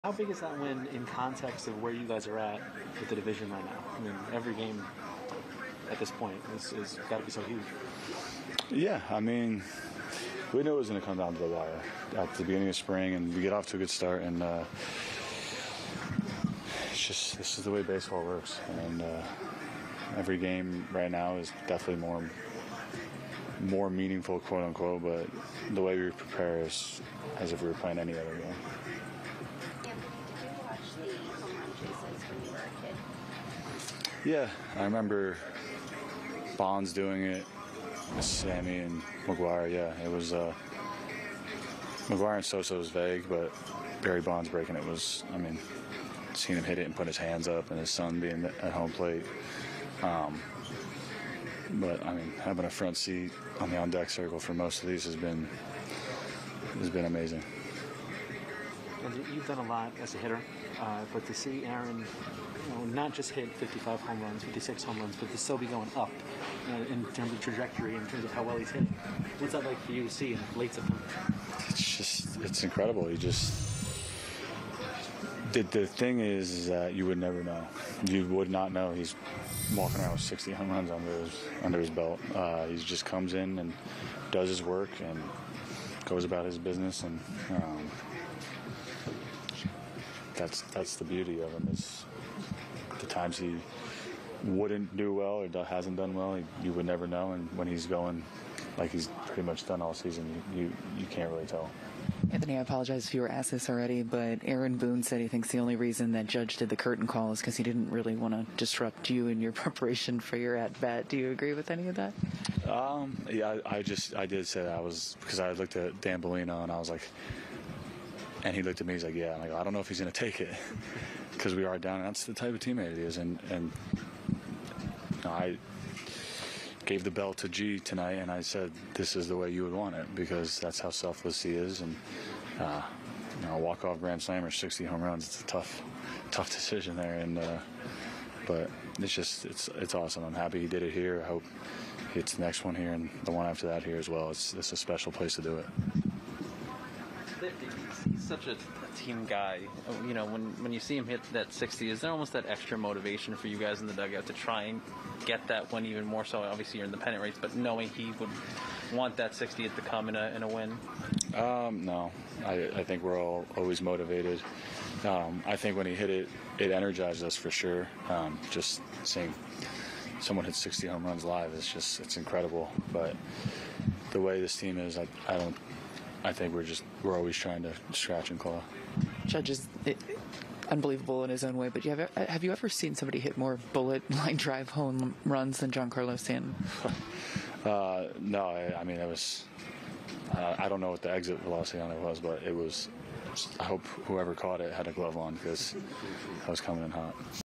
Anthony Rizzo doubles in win over Pirates
Anthony Rizzo extended his hitting streak to five games Tuesday night, including a ninth-inning double to help keep the Yankees' hopes alive against the Pirates. Rizzo met with the media after the win.